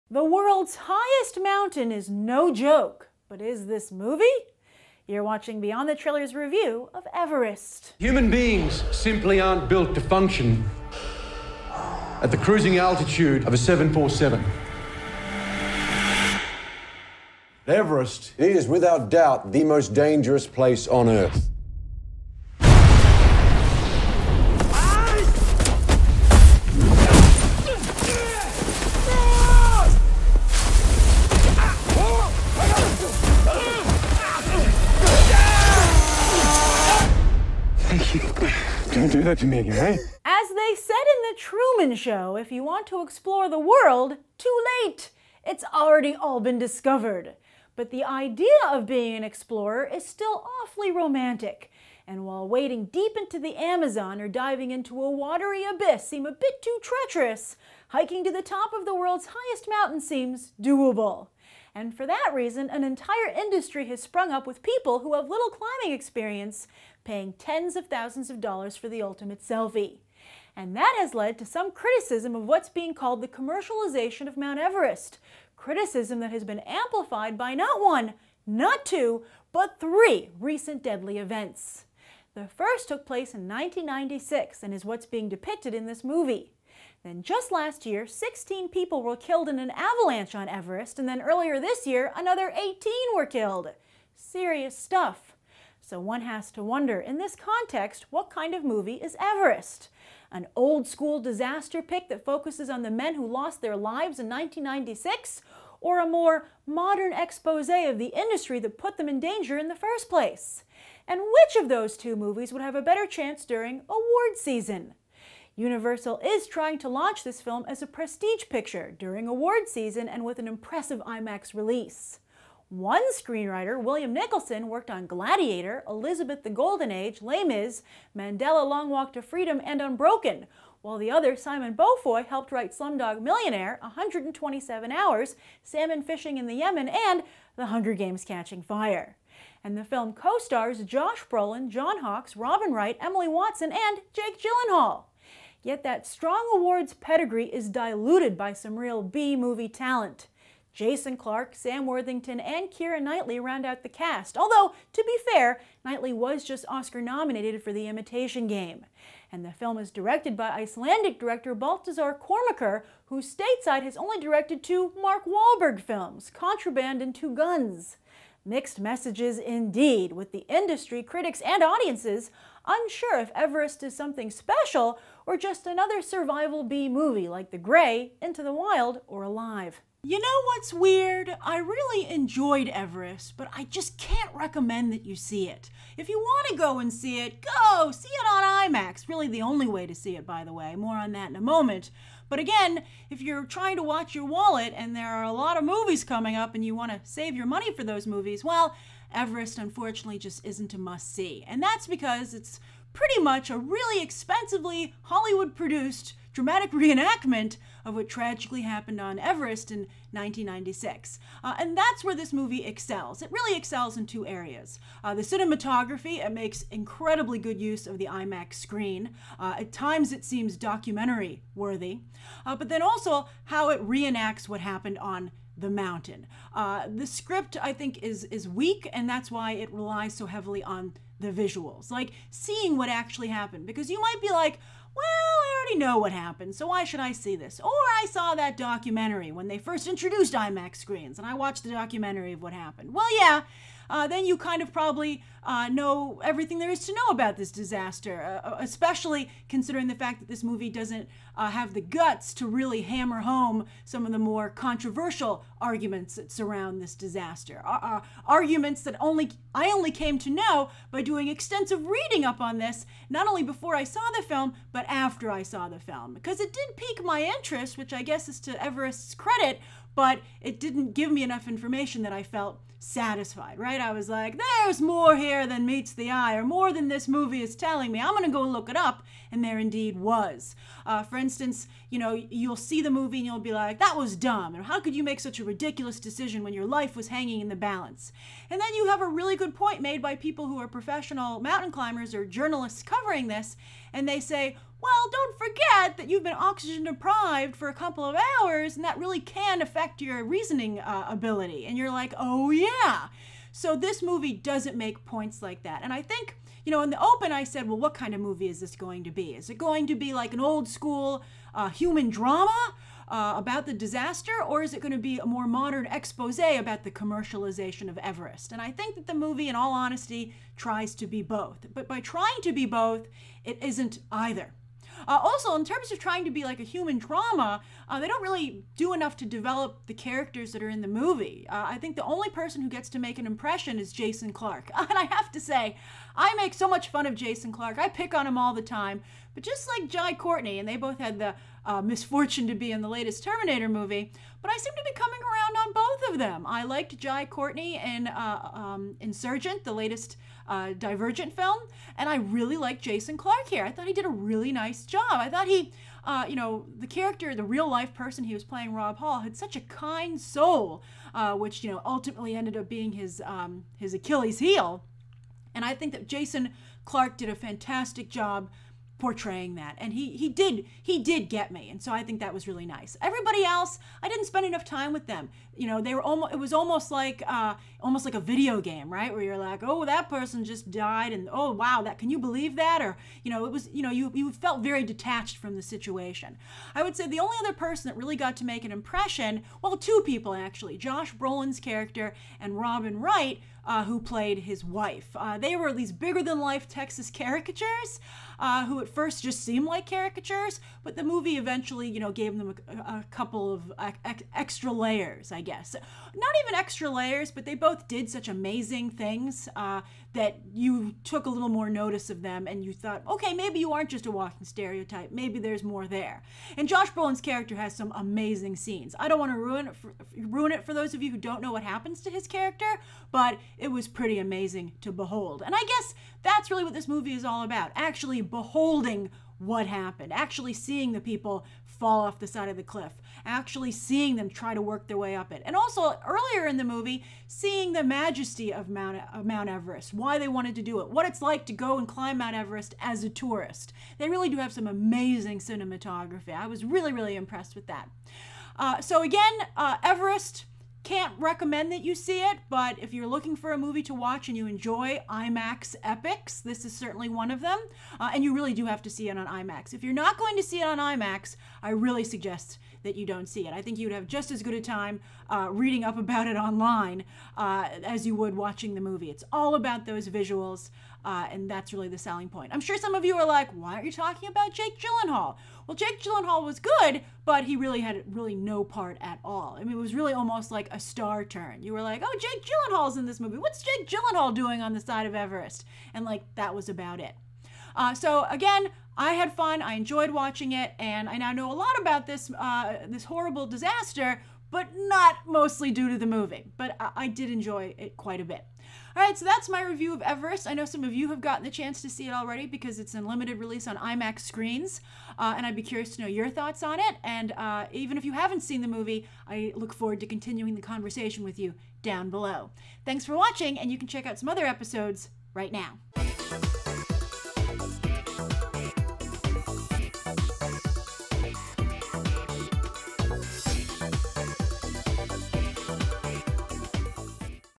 Everest 2015 Movie Review - Beyond The Trailer
Everest 2015 movie review!  Beyond The Trailer host Grace Randolph shares her review aka reaction today for this IMAX movie!

Everest 2015 Movie Review.  Beyond The Trailer host Grace Randolph gives you her own review aka reaction to Everest, the new movie about the 1996 Disaster starring Jason Clarke as Rob Hall, Josh Brolin as Beck Weathers and Jake Gyllenhaal as Scott Fischer!  Should you see the full movie?  In IMAX?  Enjoy Everest in 2015, and make Beyond The Trailer your first stop for movie news, trailer and review on YouTube today!